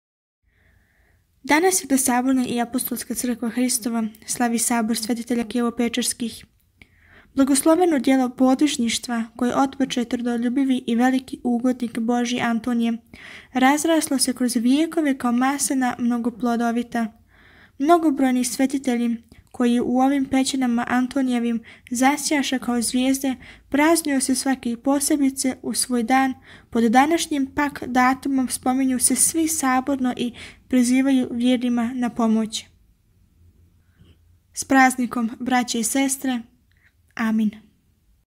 Житије Светих - Сабор Светитеља Кијево-Печерских
Данас Света Саборна и Апостолска црква Христова слави Сабор Светитеља Кијево-Печерских

С Празником

"“Охридски Пролог” Св. Владике Николаја страна 618